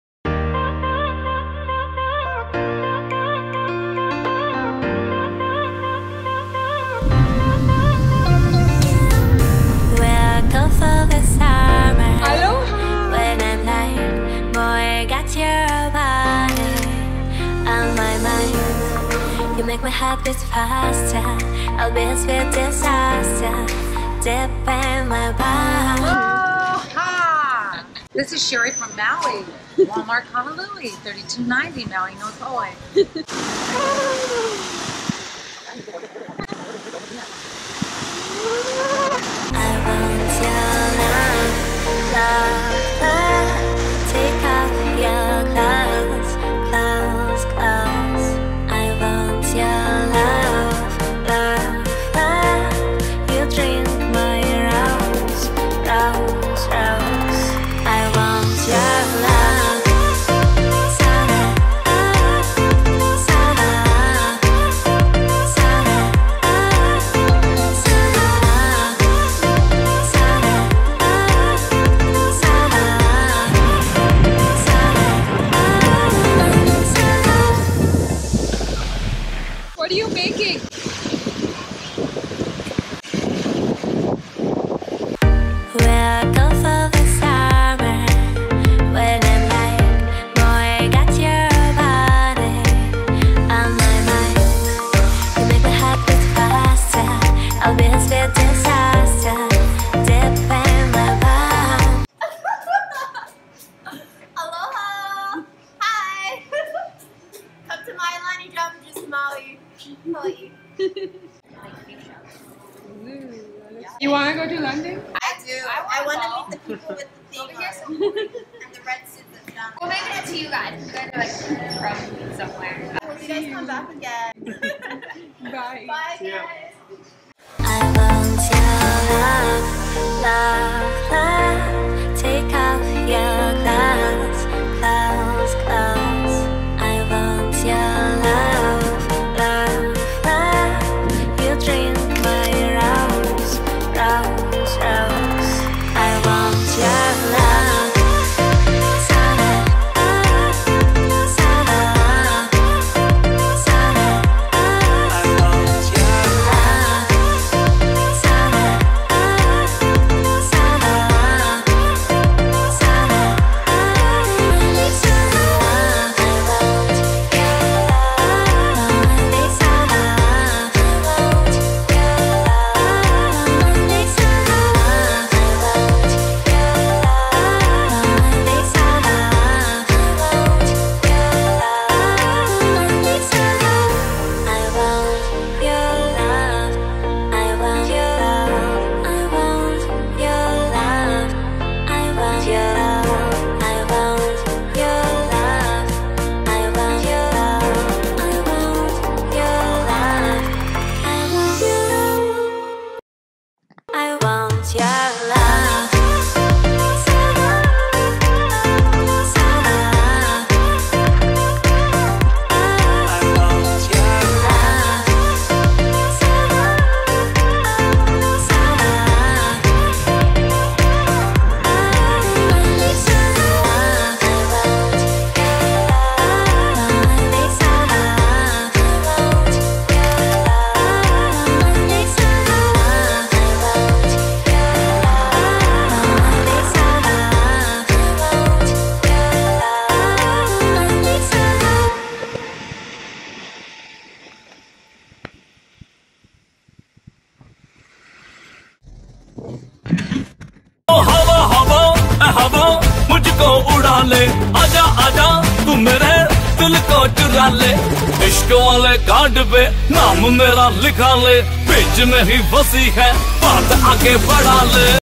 Maui, Hawaii | Trip to Hawaii (Hawaii Travel Guide)
My trip to Hawaii. Maui is the first island I visited in Hawaii. So here's Hawaii's Maui through my eyes!

Maui Travel/Vacation Guide, MUST VISITS:
Haleakalā National Park, Haleakalā sunrise, Road to Hana sightseeing, Iao valley, Maui Molokini snorkelling, Black/Red/Green sand beaches, Maui tropical plantation, and much more!